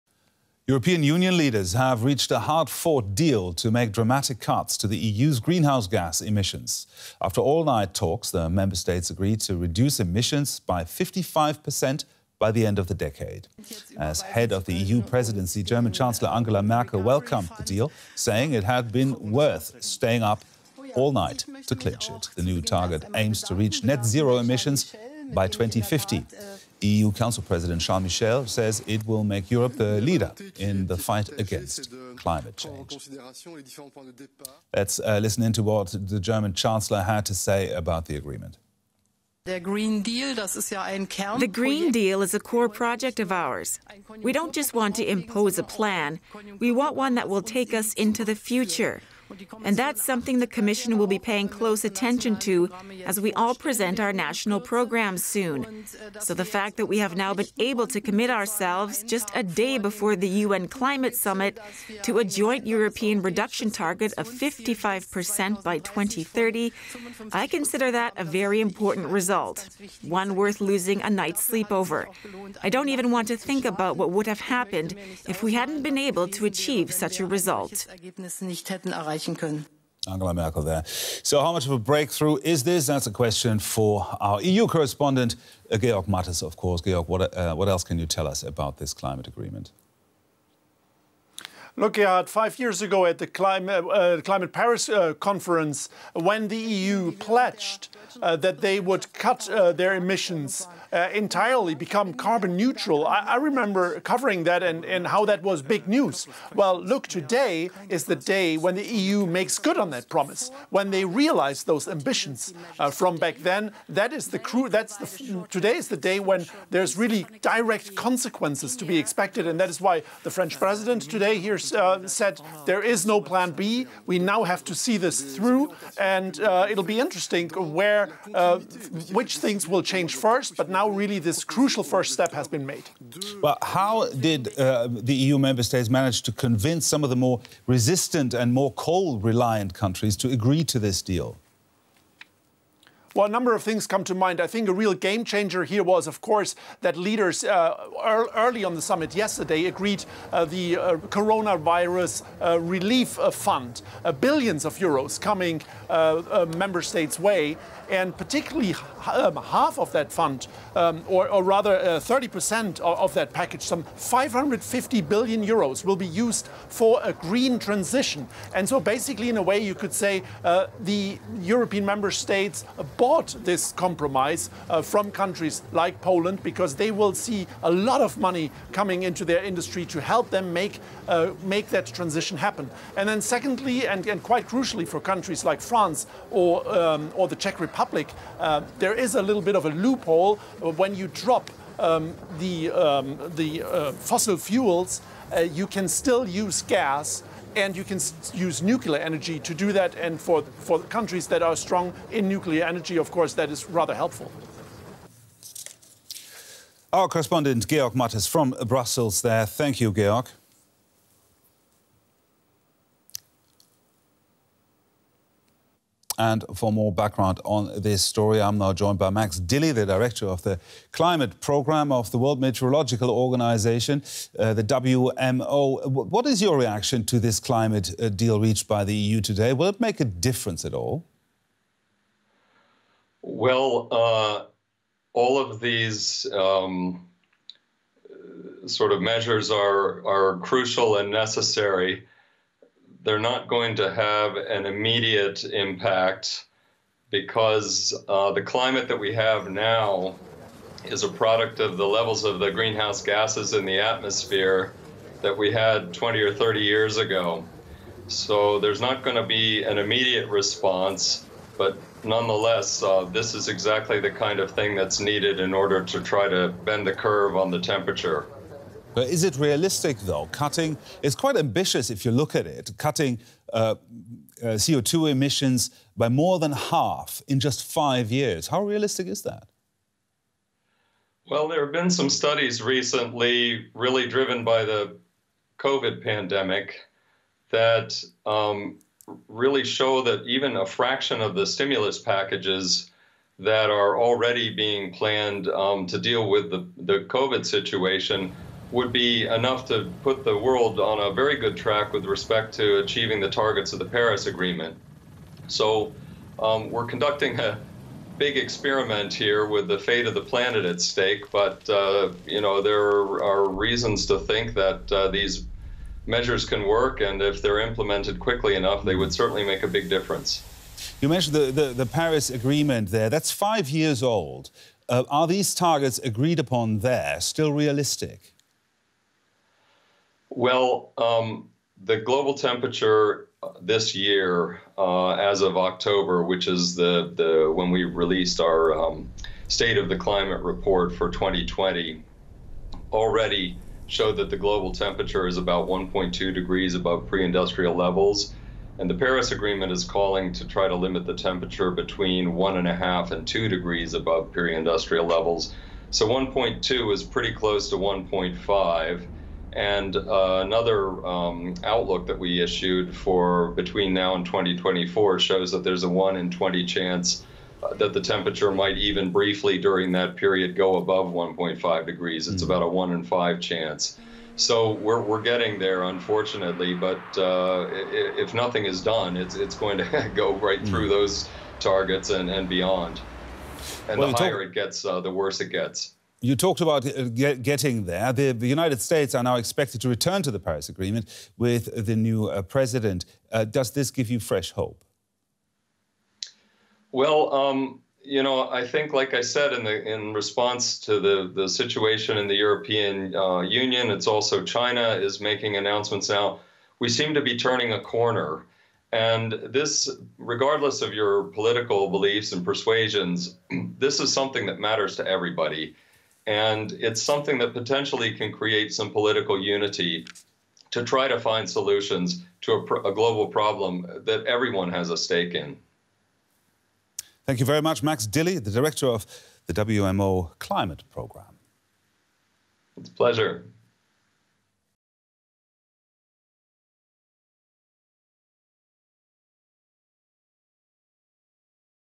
European Union agrees on Green Deal to curb emmissions | DW News
EU leaders agreed on Friday to set an ambitious target to cut greenhouse gas emissions by 55% compared to 1990 levels by 2030. The deal came after more than 10 hours of negotiations late Thursday into Friday morning. The increase from an earlier reduction target of 40% was proposed by the European Commission in September, but was met with resistance in some EU states. EU greenhouse gas emissions dropped by 24% between 1990 and 2019, according to the Commission. The economy, meanwhile, grew around 60% during the same period. The new plan will require major overhauls of the energy and transport sectors as well as a huge push to renovate and retrofit buildings to make them energy-efficient and able to charge electric vehicles.
Massive investments will be needed to help those countries with a higher reliance on fossil fuel to manage the transition, EU officials have said. Announcing the plan, European Commission President Ursula von der Leyen. She said it "puts us on a clear path towards climate neutrality in 2050."